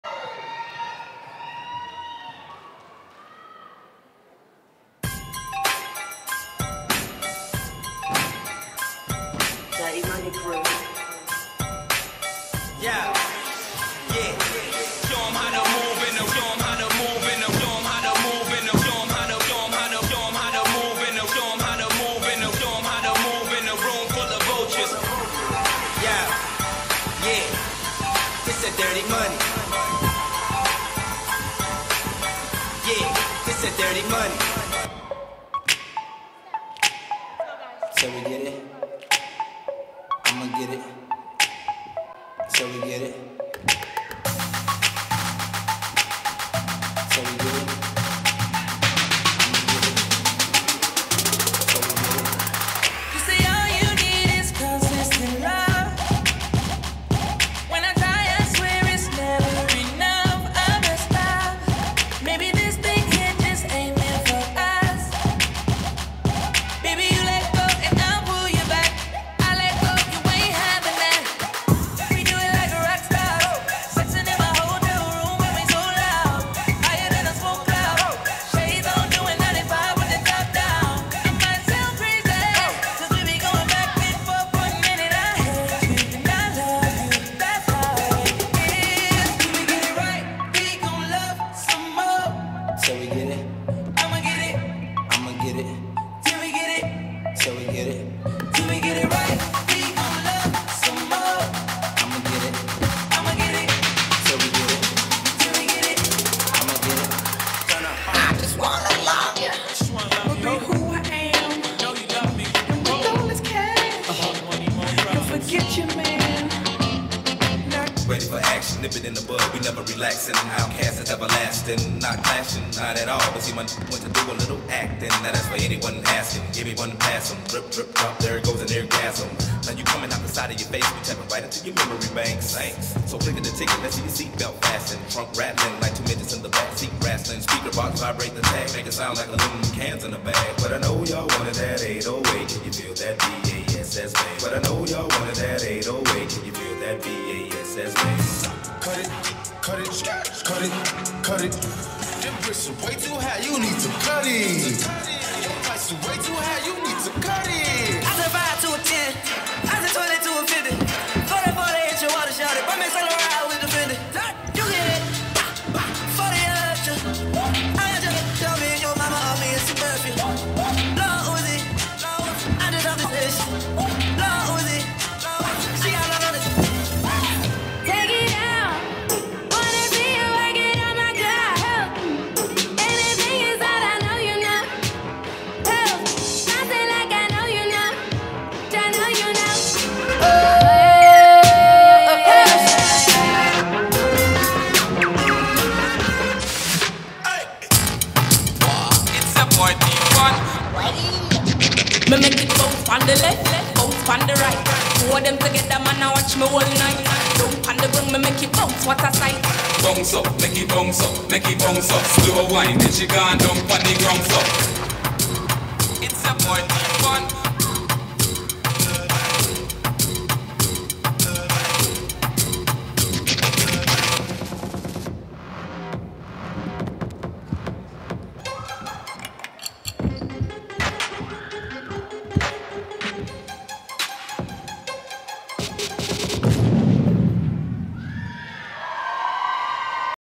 Ani Rosen, Sydney Rosen - DP Beast - Dance Precisions 2016
Dance Precisions
Sydney & Ani Rosen
Choreography by Wildabeast
Adrenaline Nationals 2016, Henderson, NV